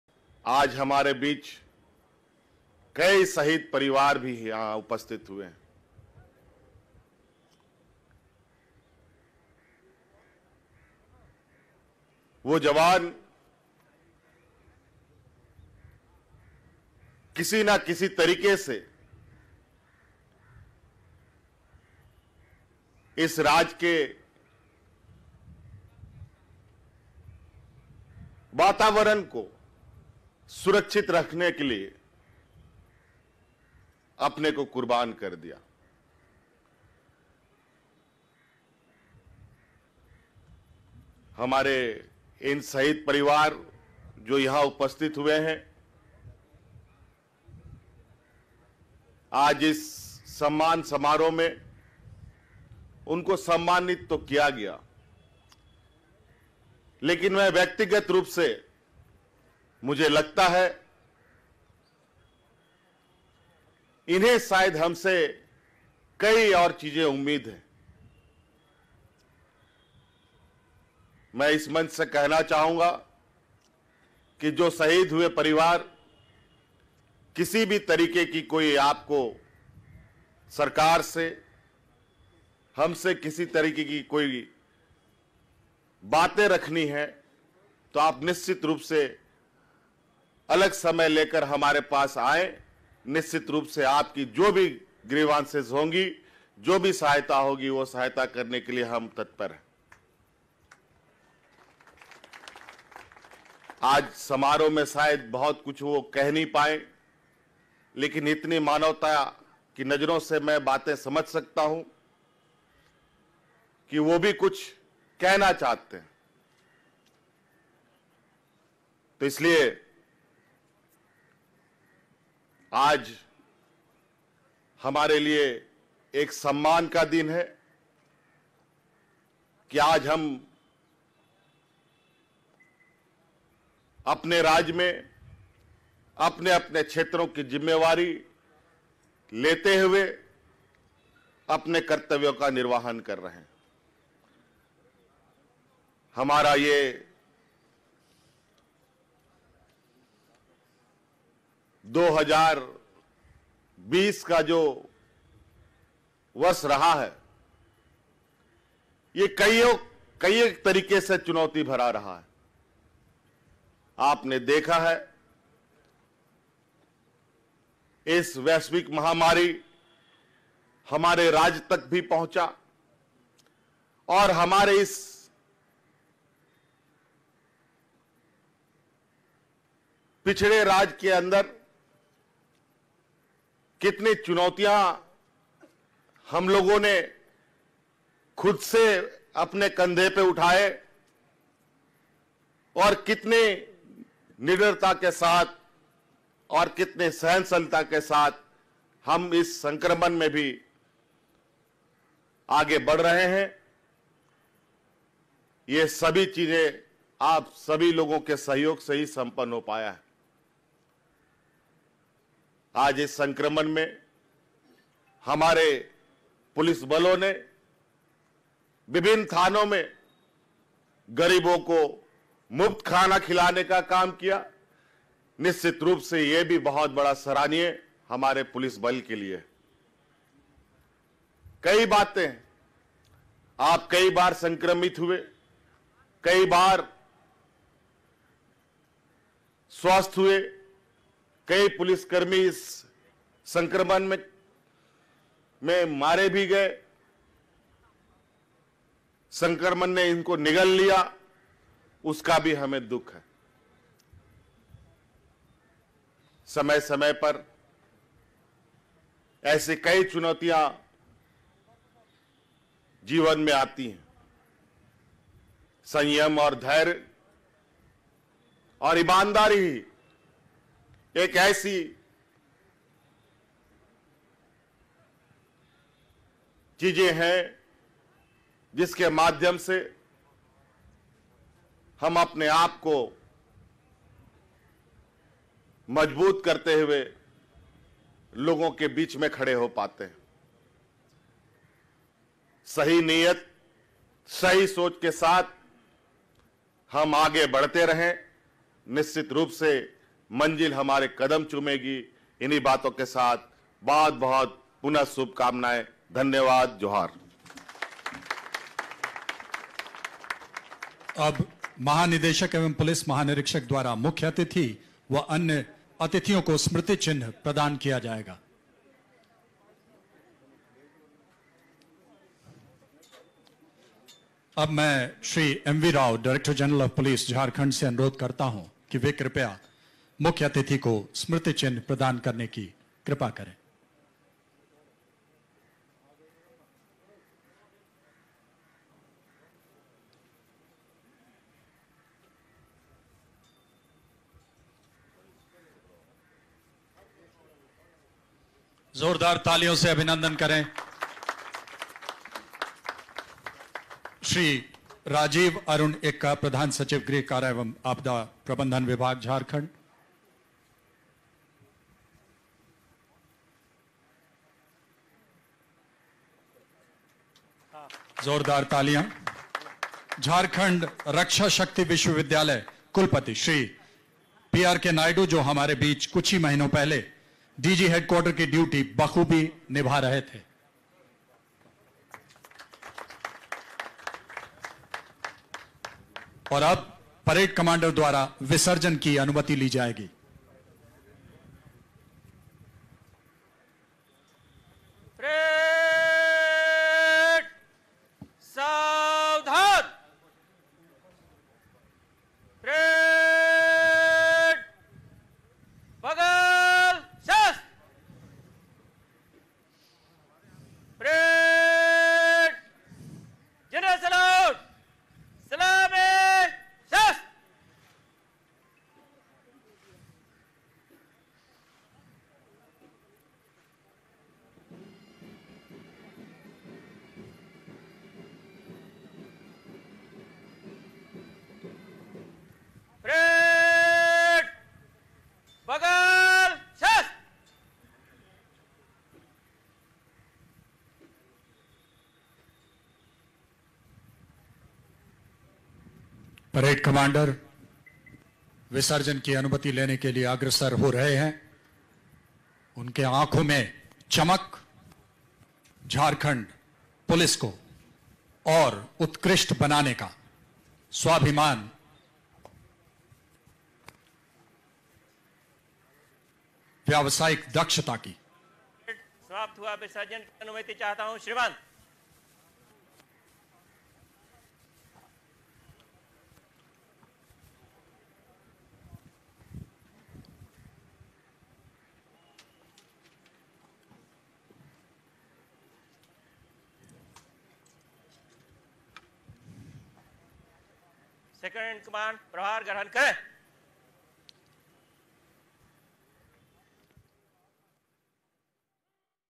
झारखंड में 05 लाख रोजगार के अवसर झारखंड स्थापना दिवस पर मुख्यमंत्री हेमंत सोरेन jharkhand news
Jharkhand 05 lakh vacancy, cm hemant Soren